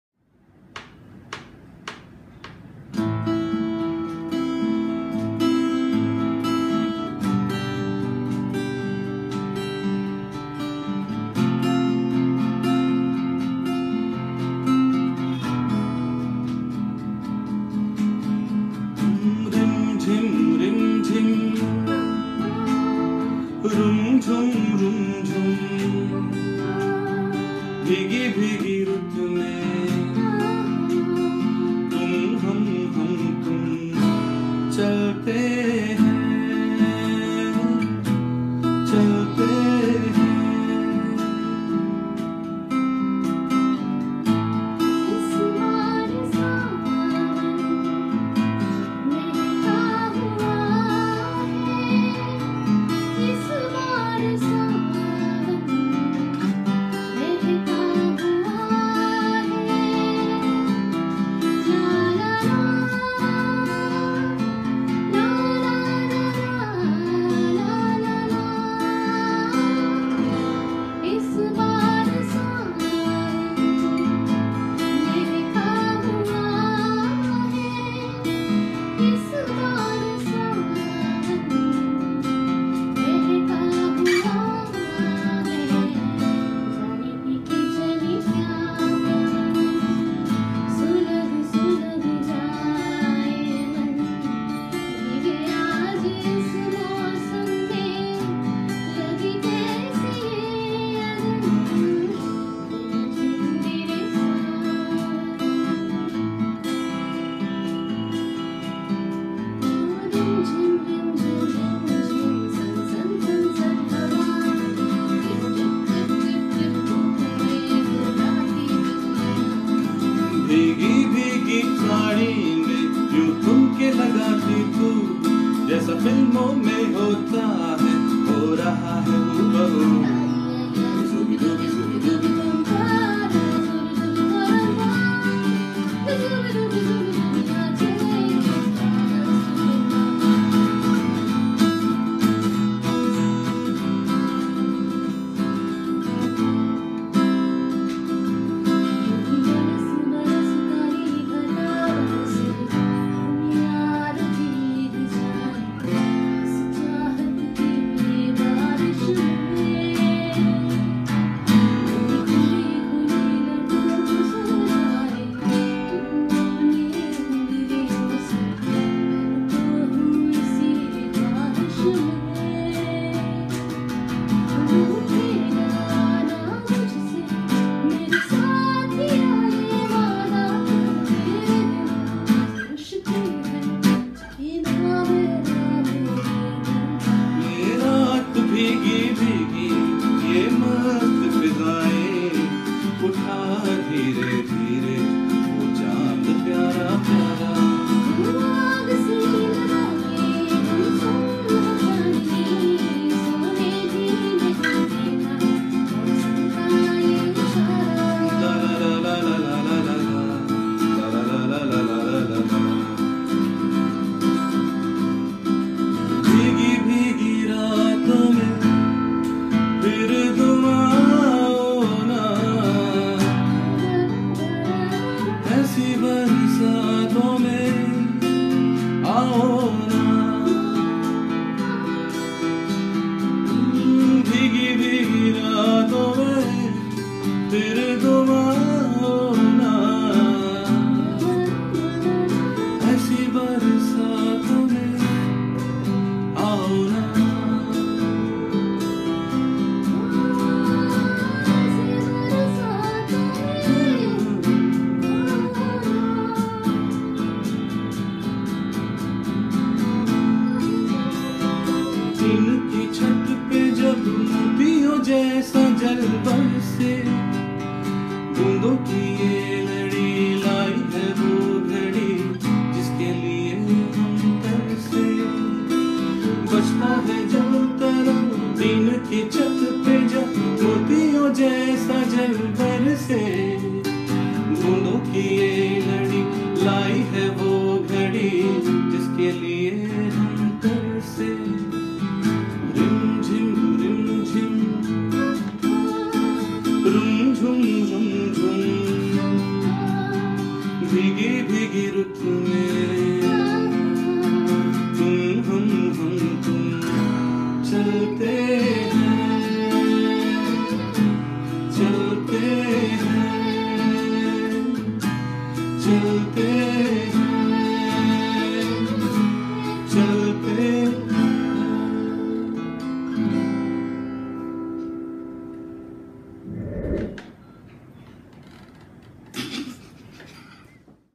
Barish waale gaane ⛈☔️😊 "Rains" special medley by Arunima and Aditya
A candid medley of some Bollywood "rainy" classics :) with my sis Arunima 💦⛈☔️😊